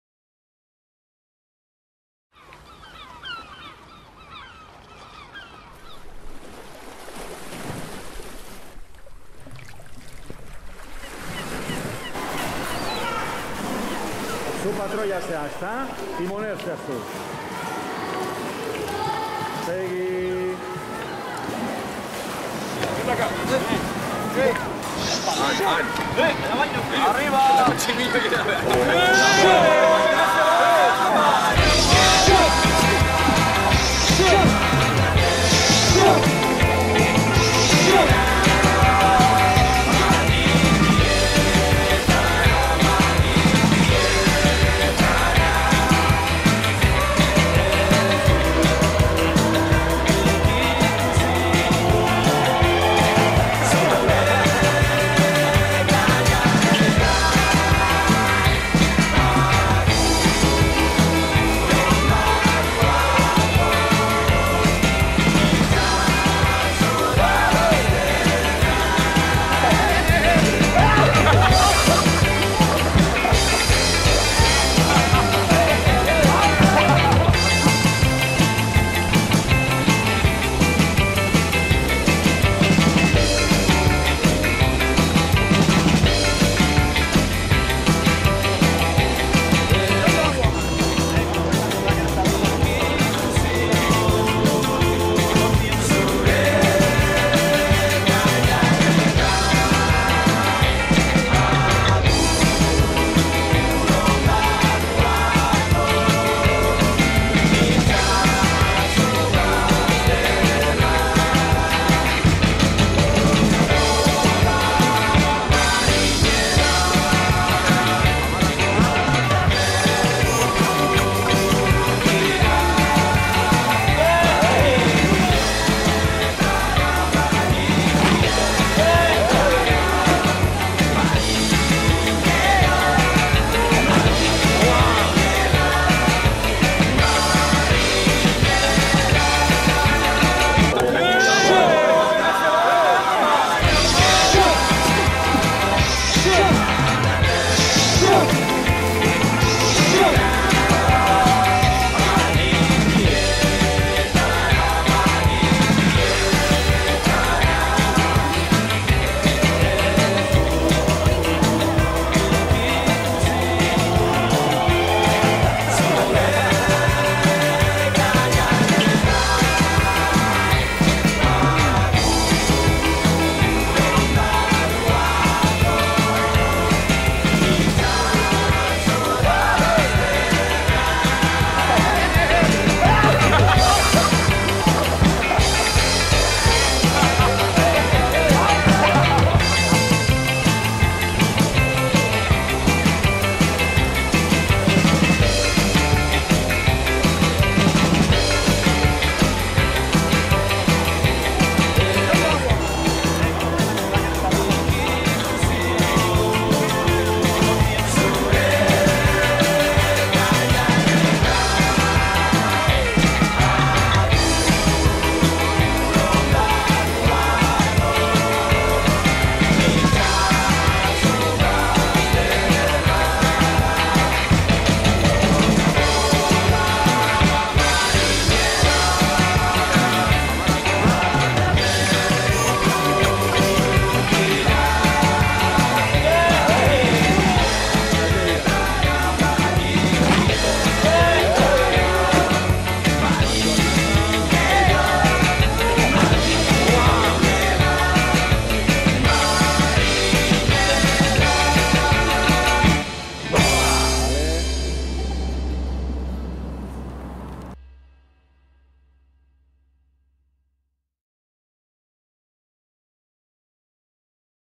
MRE - KIROLAK LH5 Ur Kirolak
LH5eko ikasleak Urkin arraunean ibili ziren.
El alumnado de 5º de primaria probó a remar en las instalaciones de Urki